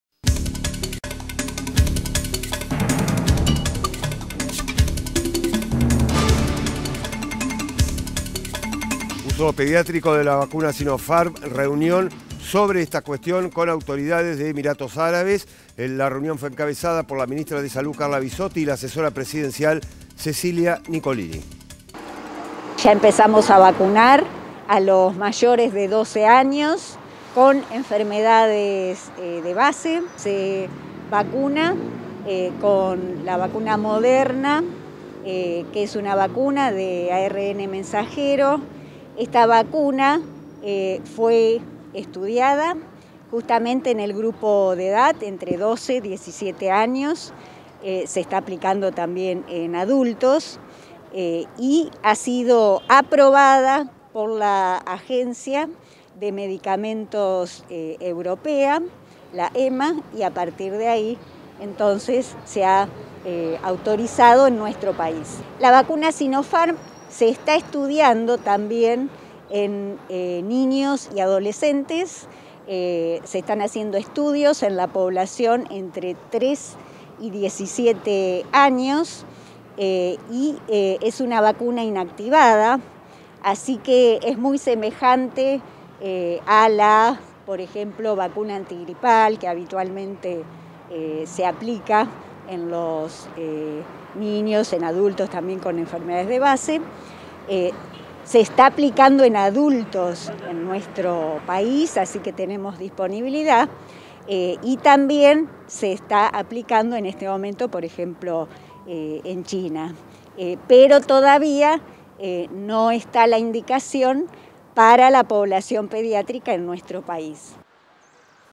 Vizzotti y Nicolini recibieron datos sobre el uso pediátrico de la vacuna Sinopharm
La ministra de Salud, Carla Vizzotti, junto con la asesora presidencial Cecilia Nicolini, mantuvo este martes una reunión virtual con investigadores y referentes del área de regulación médica de Emiratos Árabes Unidos, país que autorizó el uso de emergencia de la vacuna de Sinopharm contra la Covid-19 en personas de 3 a 17 años, se informó este martes oficialmente.